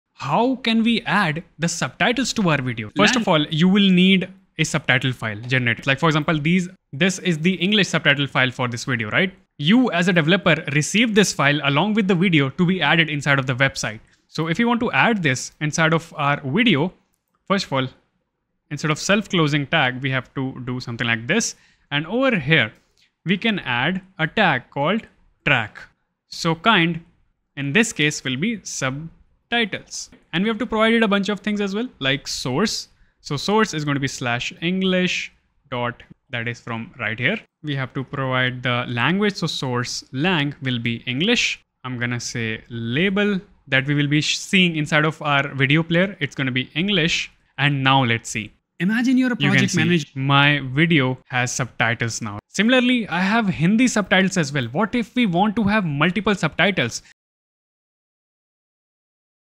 Add Subtitles to Video in React JS
Learn to build a Video Player in React JS with features like Quality Switch, Chapters, Multiple Language Subtitles, Speed, looping etc with videojs and ImageKit Adaptive Bitrate Streaming and AI Transformation. We will also understand things like hls and m3u8 streaming.